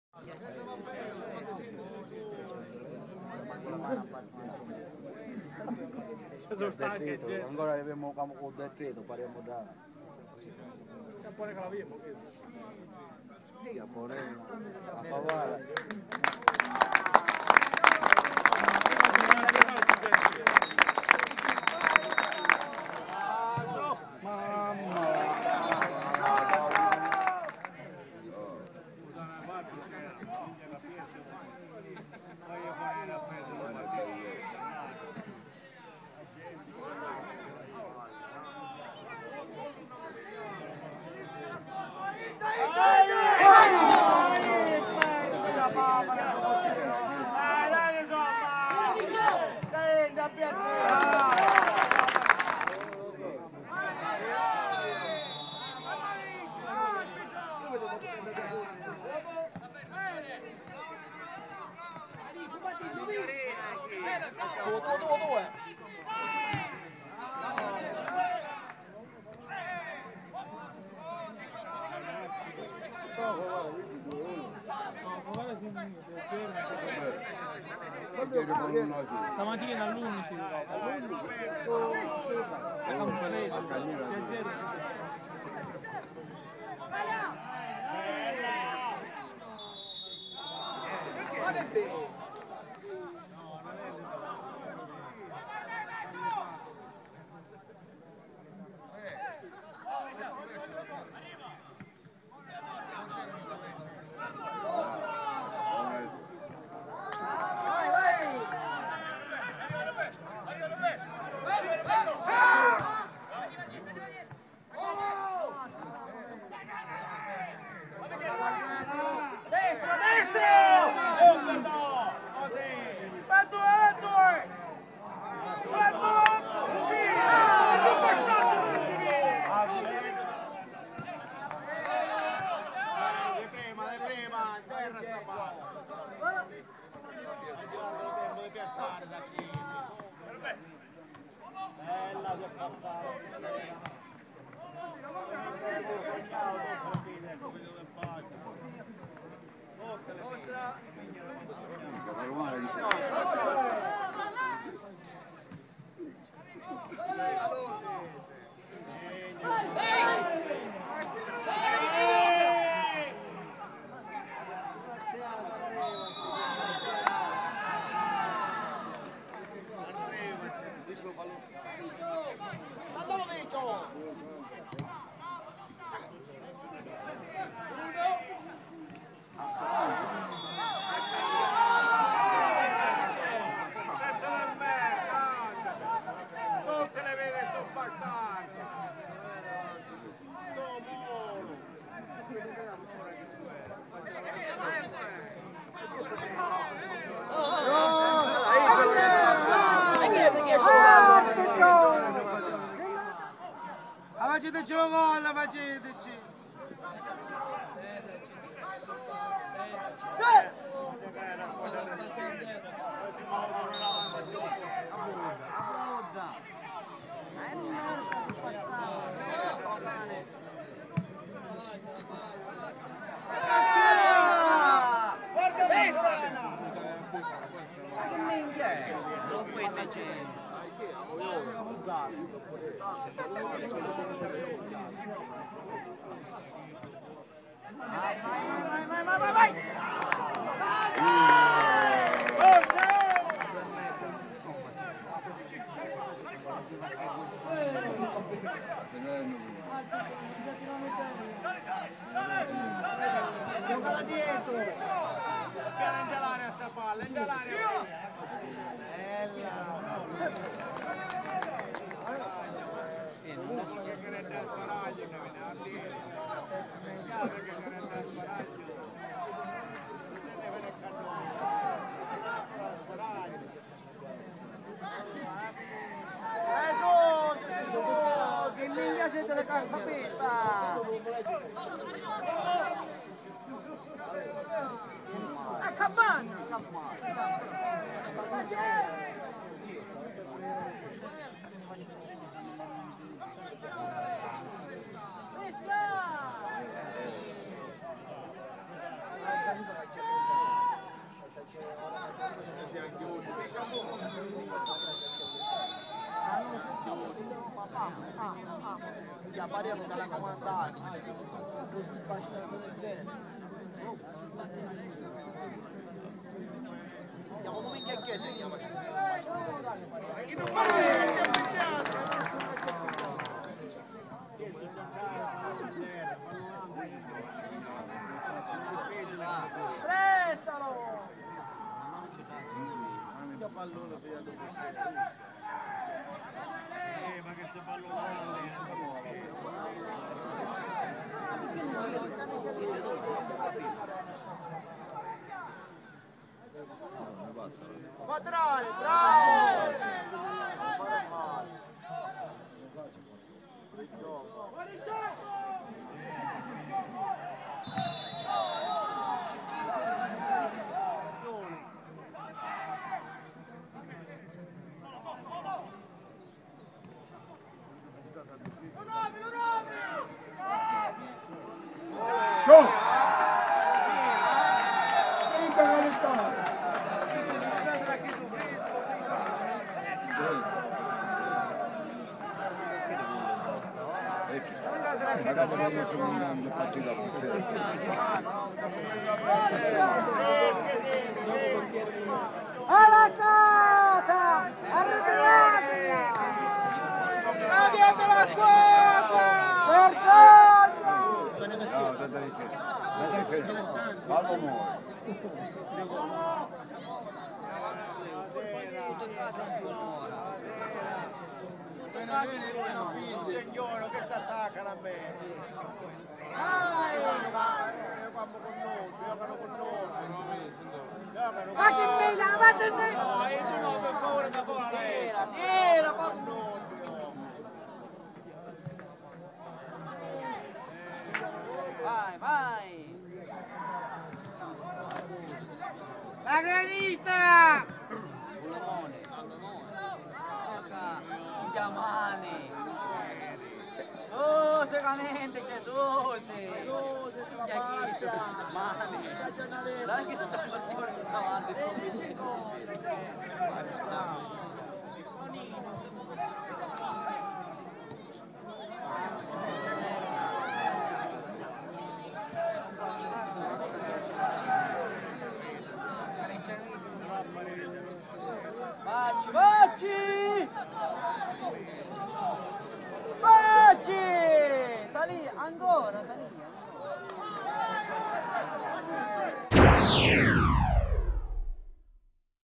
Canicatti - Sciacca 0 - 1
Lo Sciacca di Ettore Milanese vince fuori casa con il Canicattì 1-0 grazie al gol del nuovo arrivato Giuseppe Santangelo, originario di Sciacca che regala la vittoria alla squadra saccense. Un'ottima prestazione quella del giovane attaccante che con il suo ingresso rafforza notevolmente la squadra di Ettore Milanese, un Canicattì in affanno, rigore annullato al secondo tempo.

"Immensa soddisfazione in casa verde nero e dai tifosi - giungono a fine gara le parole del responsabile delle comunicazioni Baldassano - uno Sciacca spavaldo e determinato che fa ben sperare in questo campionato di Promozione".